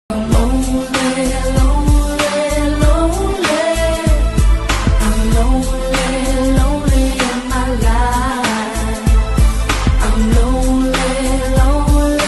lonely gorilla zoo
Hello, fellas from all around the world 🙋‍♂️
Sometimes i feel lonely like gorilla in this video 🎞️💻
There are ups and down in my life like in everyone's else. I like to express my emotions through music 🎶 That's how i came up with idea to create this shortvideo ▶️